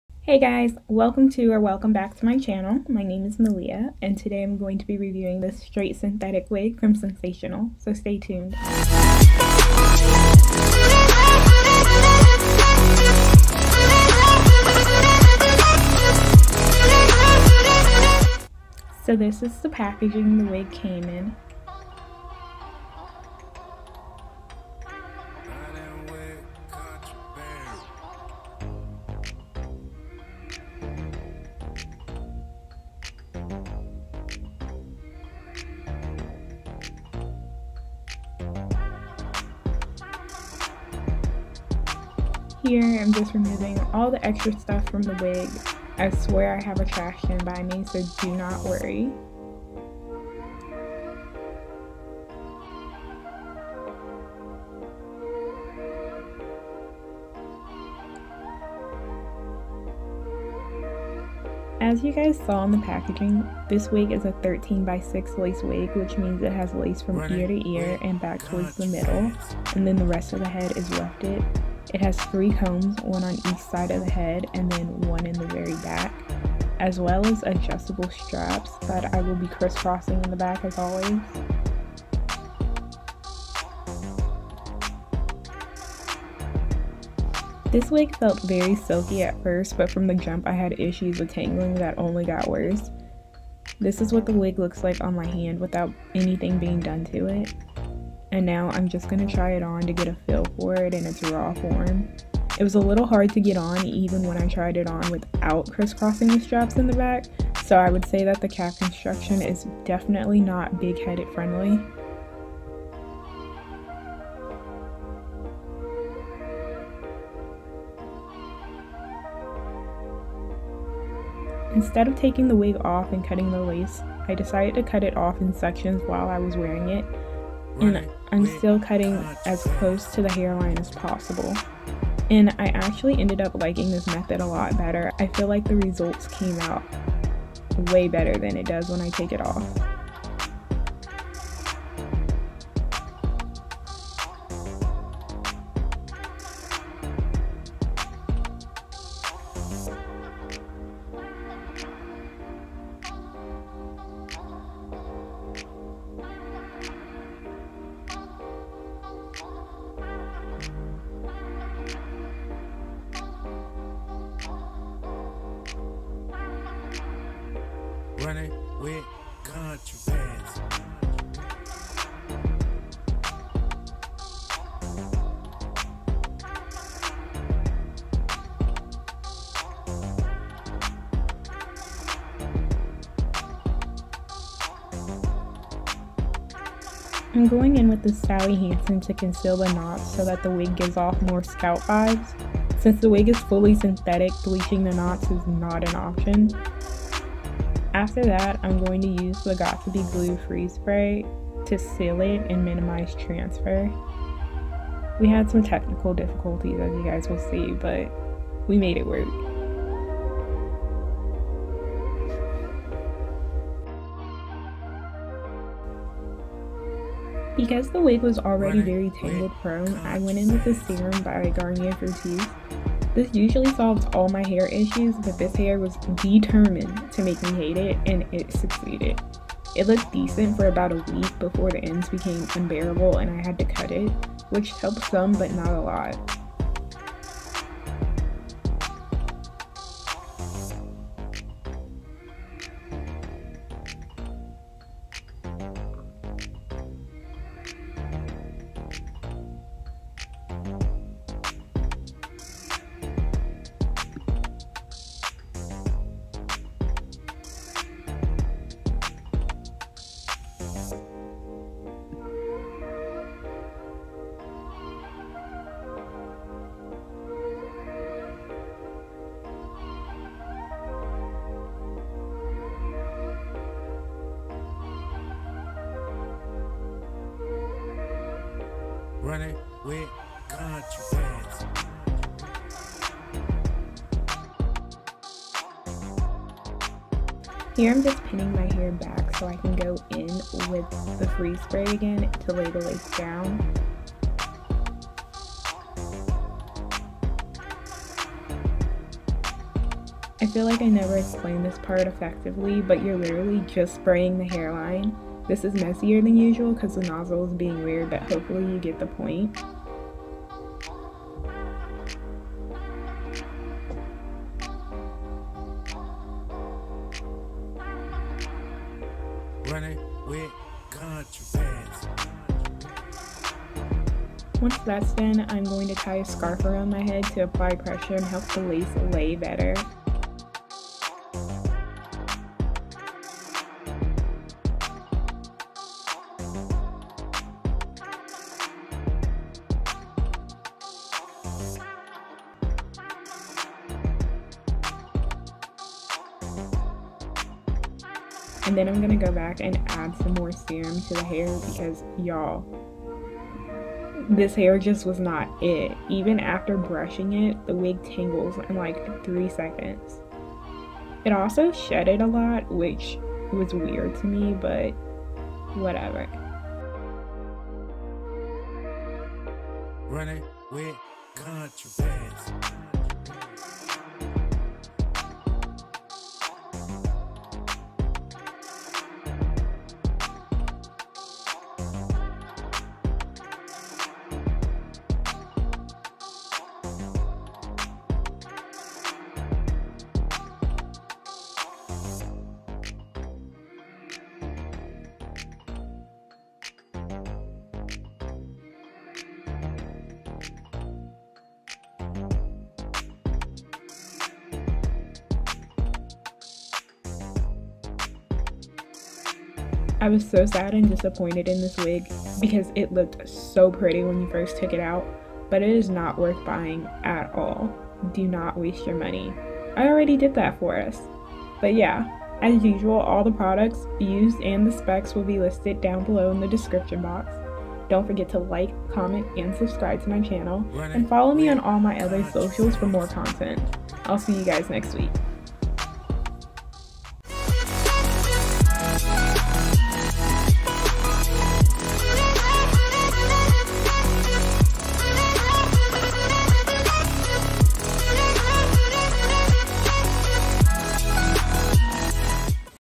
Sensationnel Synthetic Hair Lace Front Wig Review | Janelle
So. there's a first time for everything. I ordered another synthetic wig from Sensationnel and I was utterly disappointed. Every wig I have bought from them has been great except this one. You win some and you lose some. Here's a quick review of the hair when I first opened it and my experience with it. Super short like the longevity of this wig 🙃.

                                           ♡ HAIR SPECS ♡

Sensationnel Synthetic Hair Lace Front Wig Cloud 9 What Lace Swiss Lace 13X6

Style: Janelle

COLOR: 1B

Density: Natural

Pre-plucked

Baby Hair

Price: $49.99

                               ♡ P R O D U C T S   S H O W N ♡

◦ Sally Hansen Airbrush Legs - Light

◦ Got2b Glued Blasting Freeze Spray

◦ Garnier Fructis Sleek & SHine Anti-Frizz Serum

                                                         ♡ MUSIC ♡